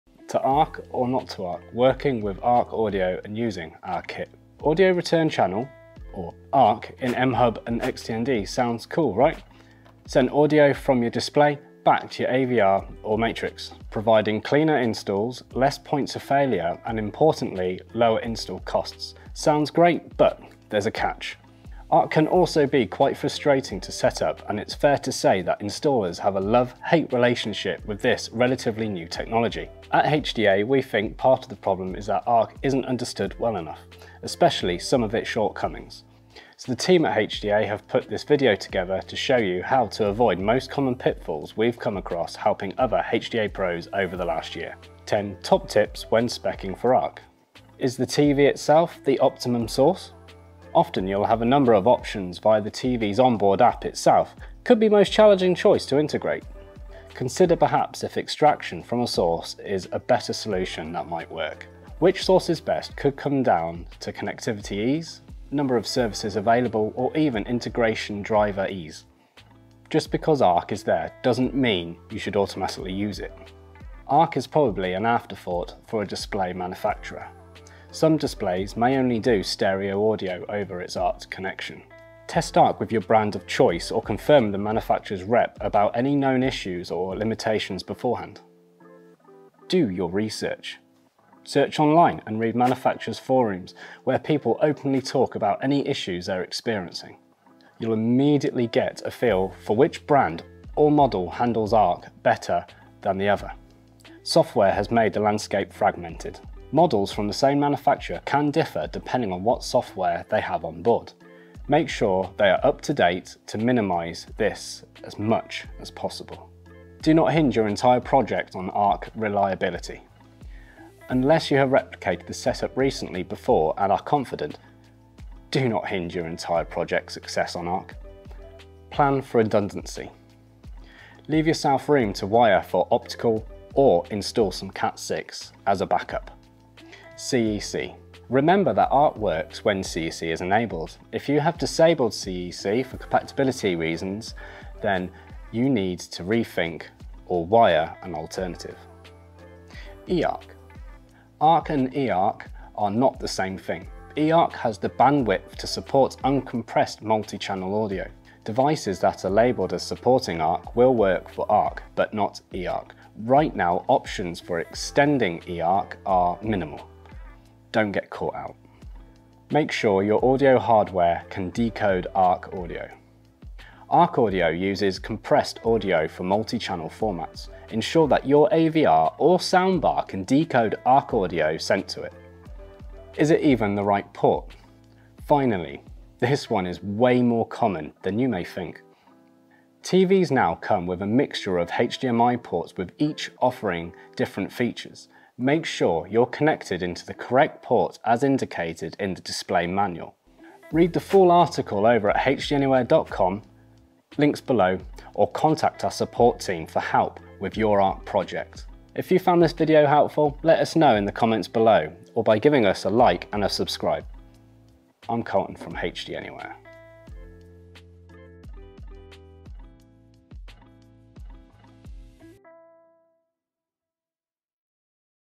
Avoid HDMI ARC pitfalls with these 10 top tips!
ARC is a new way to take audio from your TVs onboard apps (like Netflix, Disney+, Amazon Prime etc) back to an AVR or audio distribution device like MHUB using just your HDMI or network cable.

In simple setups it often works fine. But, if you're using ARC in a distributed setup or need to mix and match audio being routed to your speakers you might bump into some difficulty. This video helps to explain some of the most common pitfalls when using HDMI-ARC and how to avoid them.